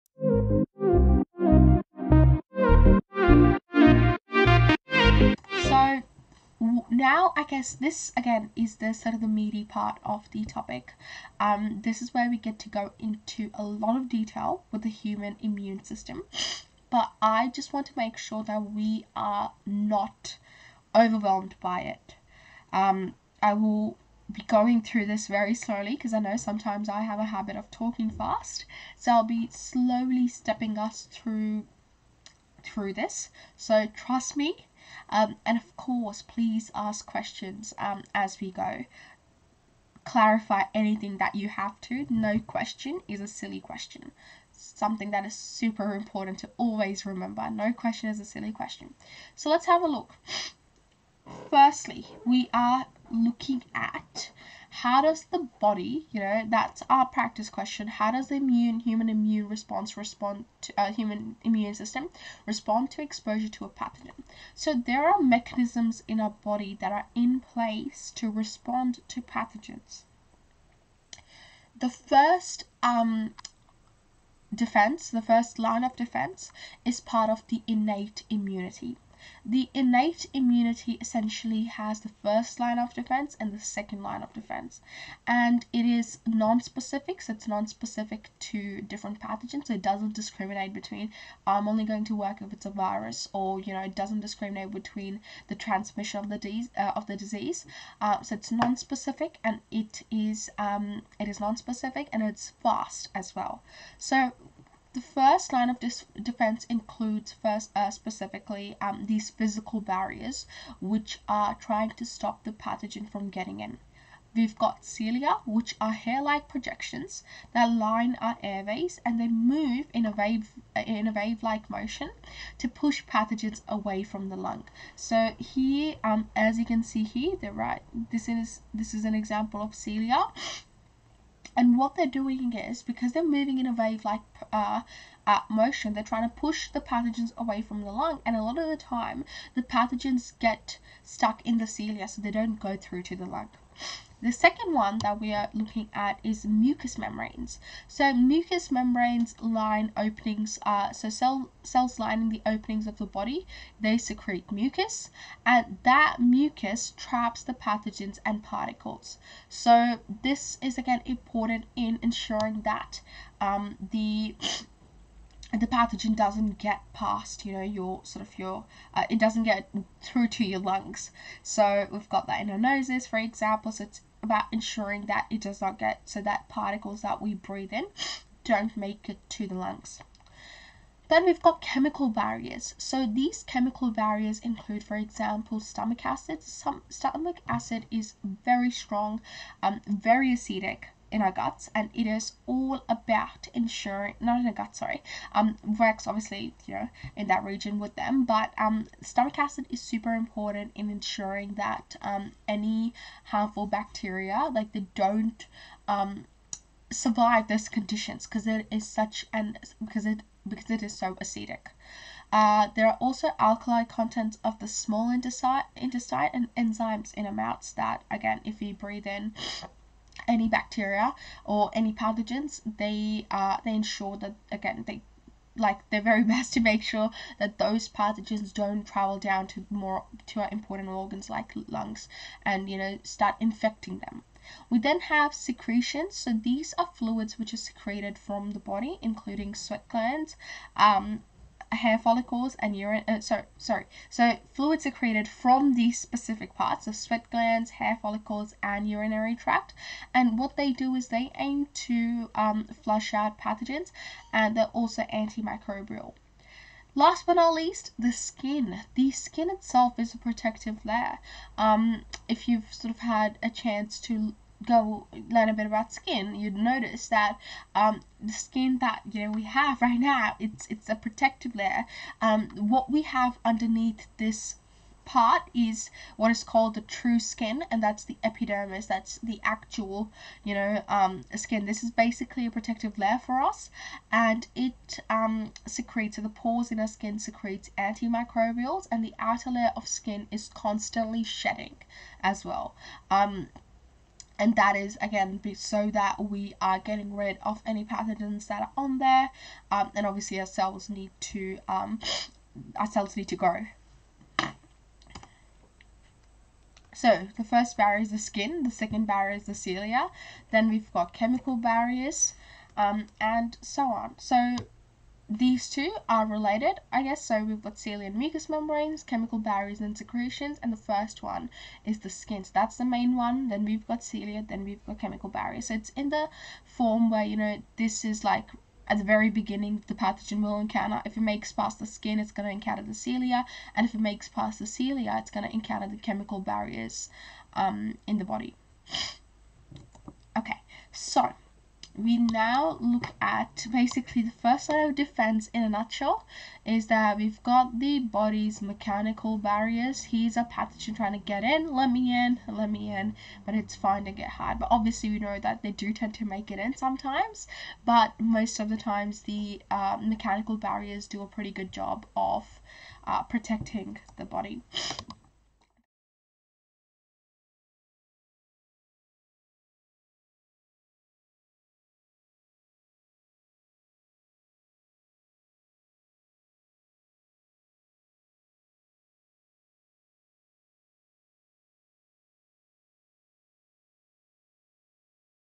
First Line of Defence Immunity | HSC Year 12 Biology
An expert summary on First Line of Defence Immunity for HSC Year 12 Biology. Covers everything you need to know including; infectious disease, immunity, protection, and more!

Want more help? ATAR Notes publishes amazing Course Notes & Topic Tests for HSC Year 12 Biology.